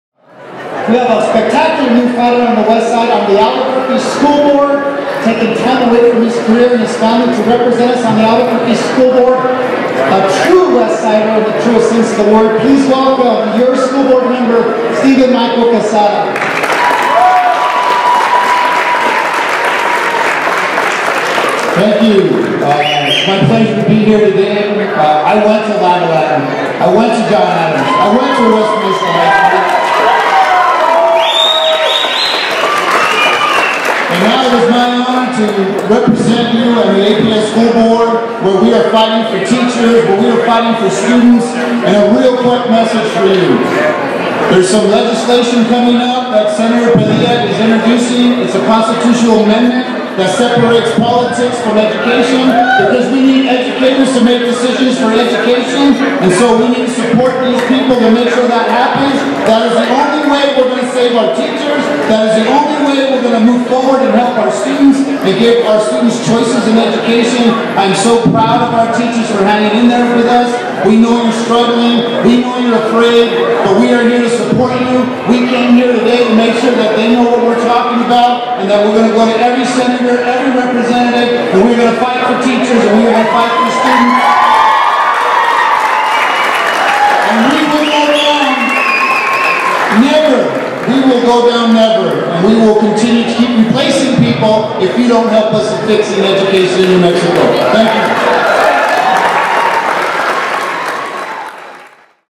New Mexico Legislature - Albuquerque West Side Day - Steven Michael Quezada
Albuquerque School Board member Steven Michael Quezada presents at Albuquerque West Side Day.